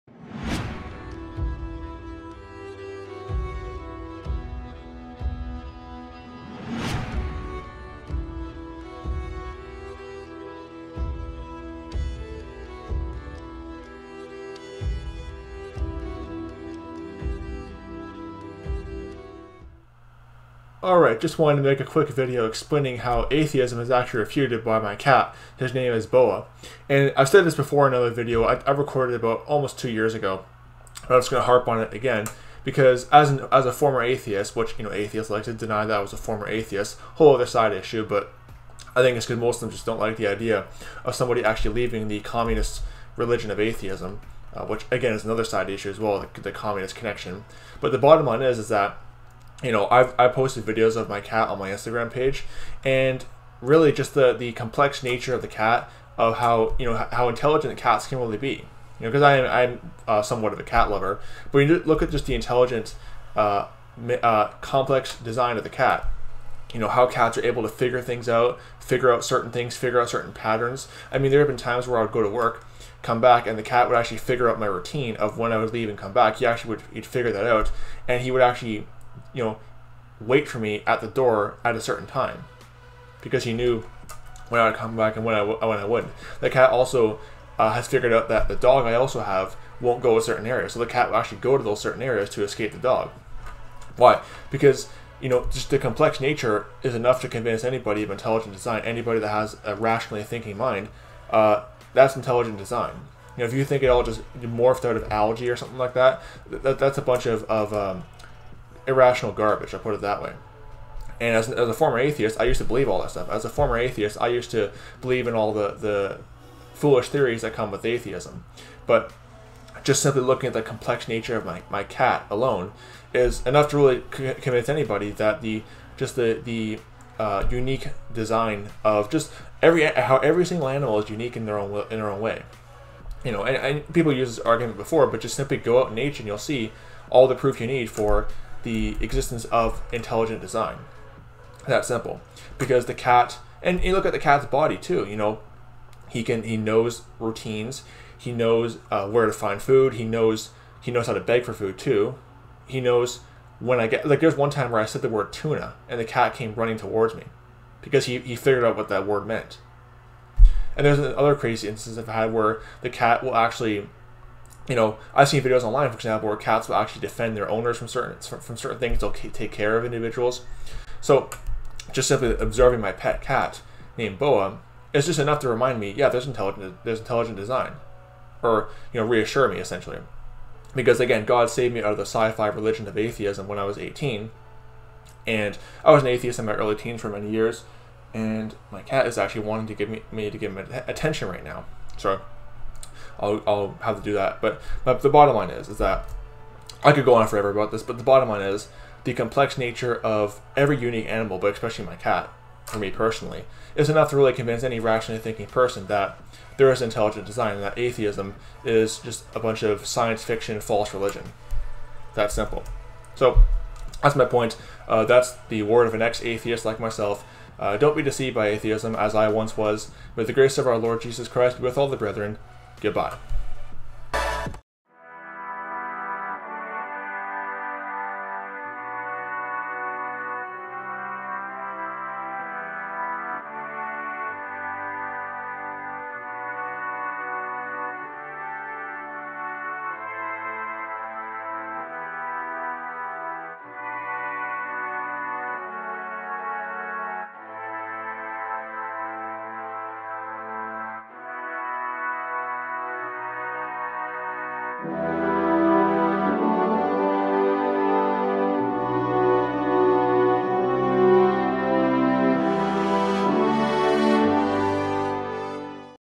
Atheism Intellectually Destroyed By My Cat
My cat (his name is Boa) rationally destroys atheism through his complex bodily functions and intelligence.